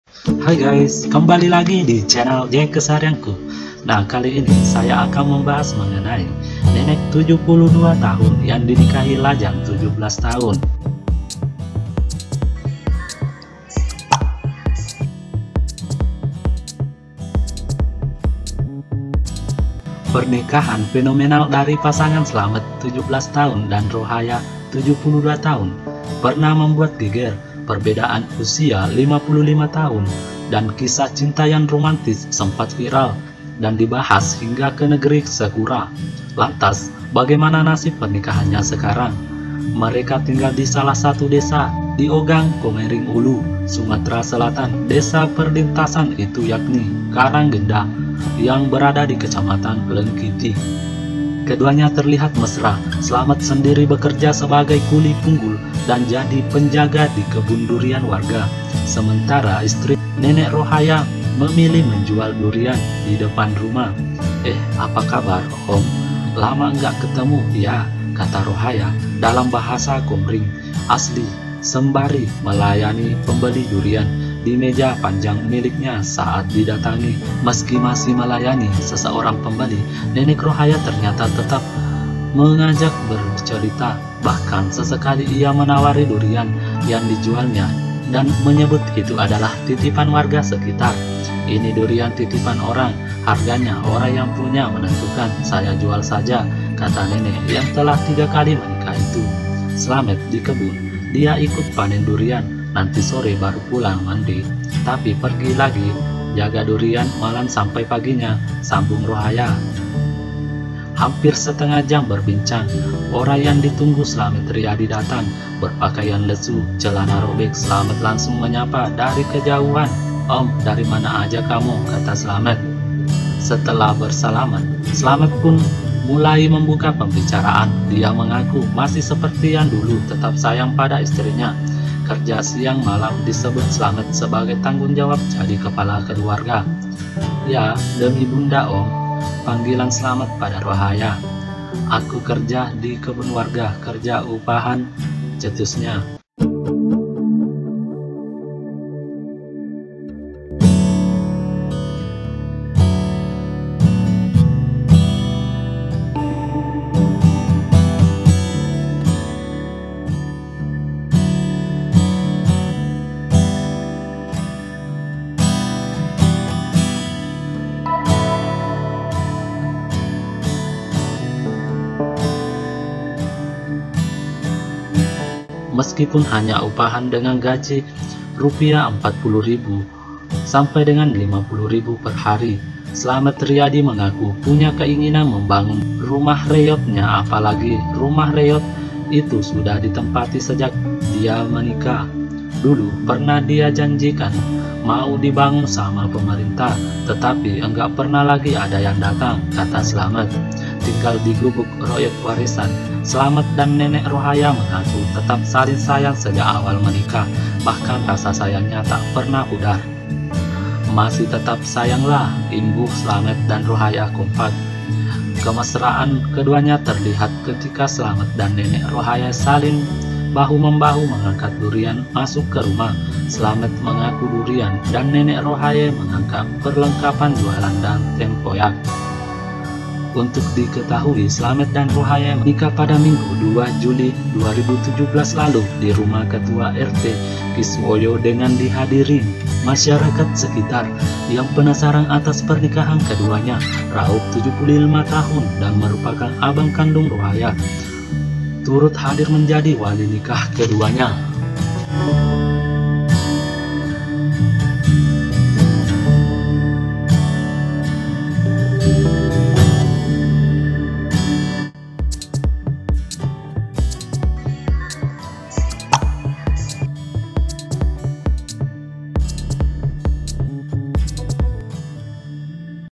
inilah NASIB Pernikahan Nenek Rohaya & Slamet
nenek rohaya umur 72 tahun dan slamet umur 17 tahun menikah dengan perbedaan usia 55 tahun, keduanyapun saling mencintai layaknya suami istri